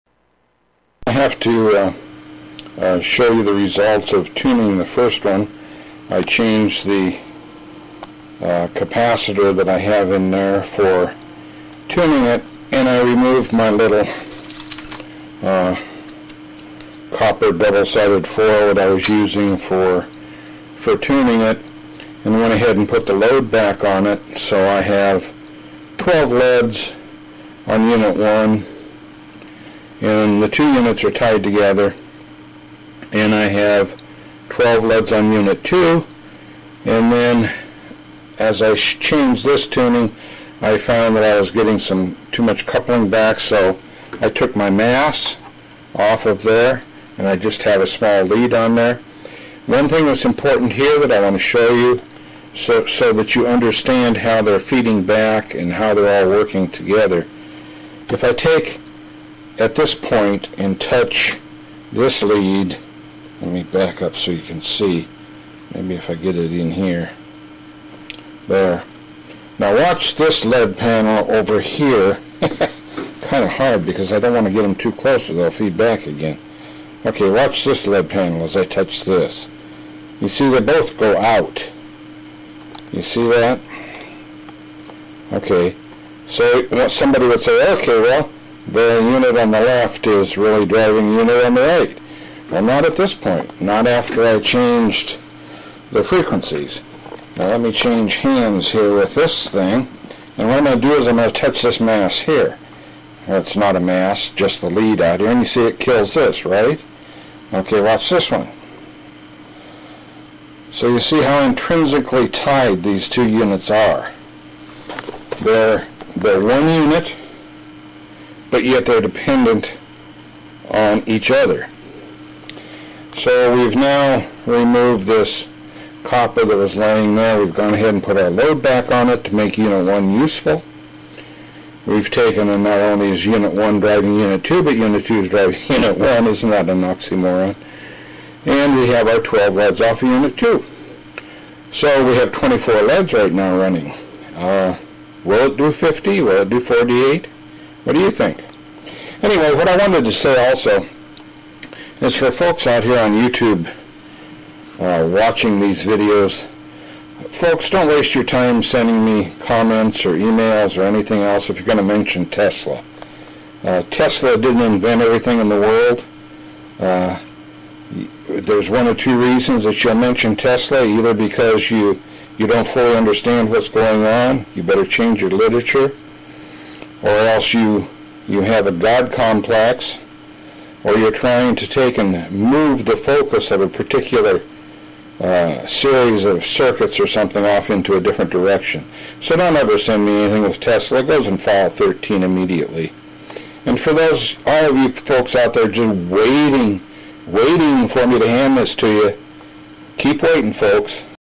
PSEC two tied gain units
Here different from the last video is the proper tuning on the left unit which now allows for the addition back of a load off L3. Now both units are able to supply a load.